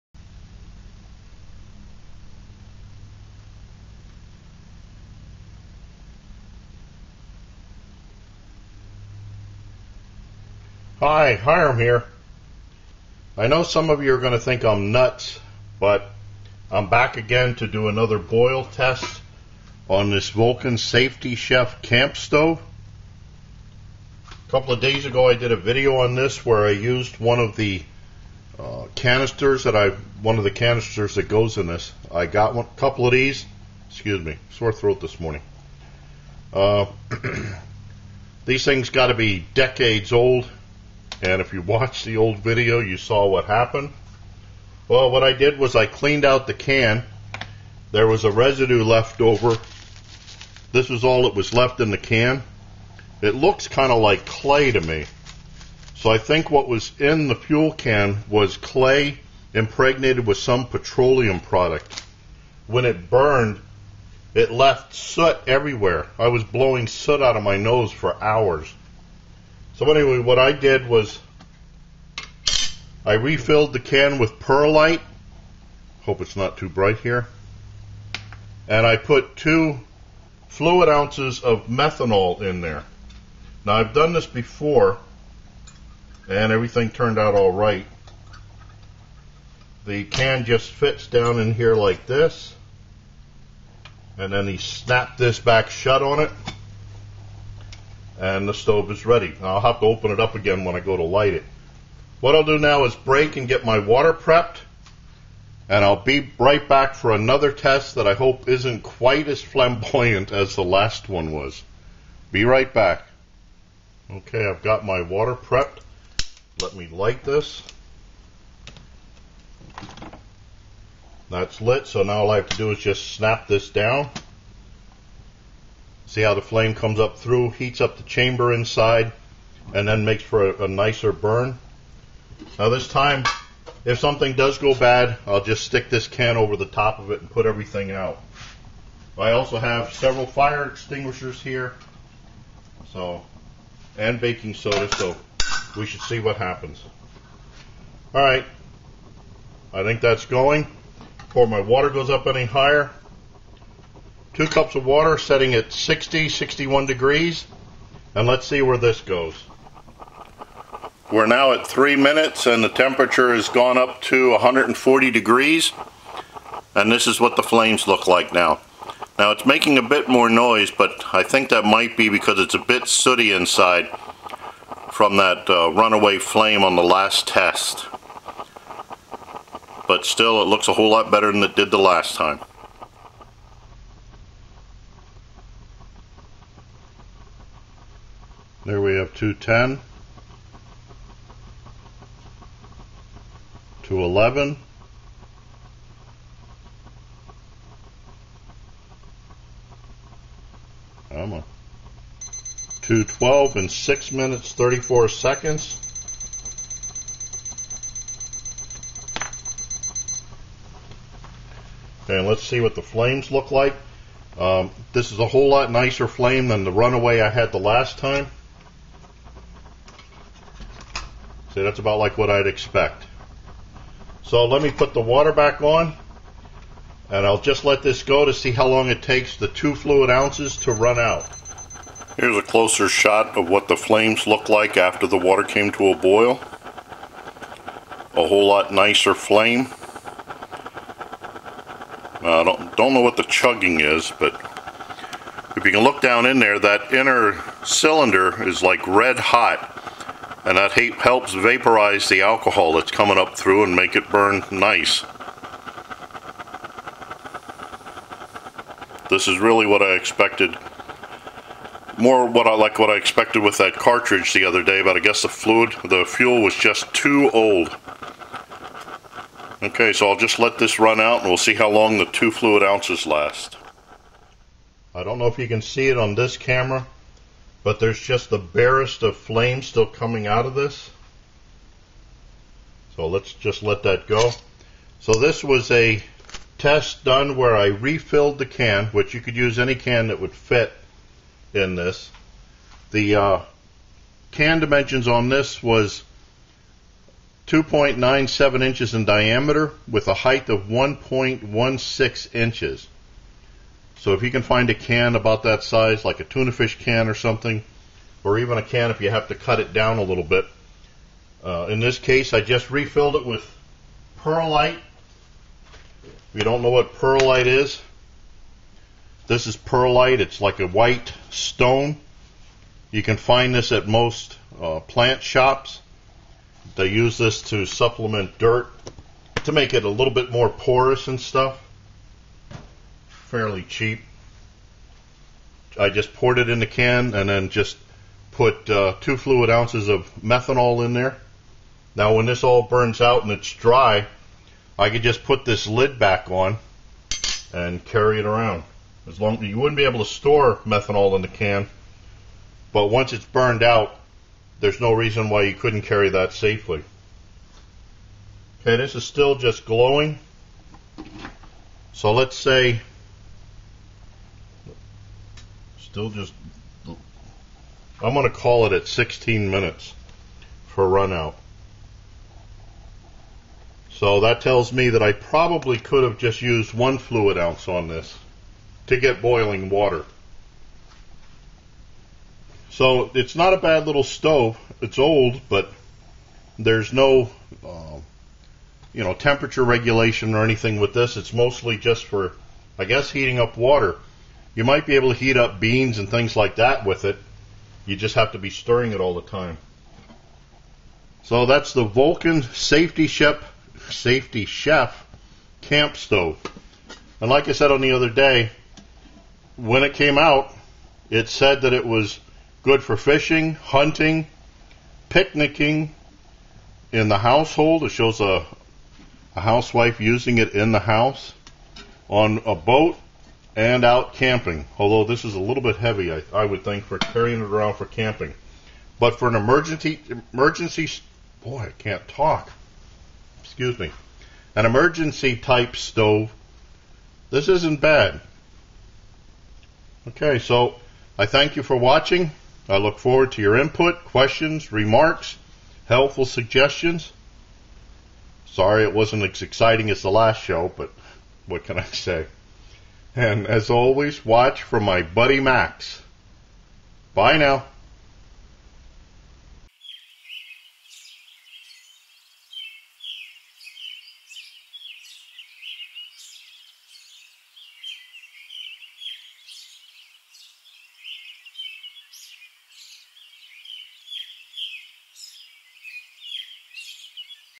Vulcan Safety Chef Camp Stove - Boil Test #2 - (Perlite & Methanol Re-fill)
The last time I tried my Vulcan Safety Chef Camp Stove with the old fuel can things got a little exciting. From past use I know the Vulcan is a decent stove so I thought I would try another video but this time I refilled the fuel can with perlite and two fluid ounces of methanol.

This time I got a much better flame and the test went well. I would suggest that if anyone has the old Vulcan fuel cans DON'T USE THEM!!

By the way I forgot to mention that the two fluid ounces of methanol did not fill up the can. A little bit more could have been added but I didn't feel it was needed for this test.

Here is a link to the more exciting test of the Vulcan Safety Chef Camp Stove. If you haven't already seen it I suggest you watch and learn. Remember, I do these videos so you don't have to. 8-)

Hiram

Lab0326

Title: Vulcan Safety Chef Camp Stove - Boil Test #2 - (Perlite & Methanol Re-fill)
Room temp: 81°
Humidity: 64%
Weather condition: Partly Cloudy
Burner: Vulcan Safety Chef Camp Stove
Stove/Pot stand: Vulcan Safety Chef Camp Stove
Pot: Boy Scout Pot #2
Fuel: Vulcan Fuel Can refilled with perlite and two fluid ounces of methanol
Amount of fuel at start: NA
Amount of water: 2 cup(s)
Water temp at start: 60°
Time to boil : 6 minute(s) 34 second(s)
Time run:  16 minute(s) 00 second(s)
Water temp reached:

Note: If you have an old can of Vulcan Fuel I suggest you don't use it.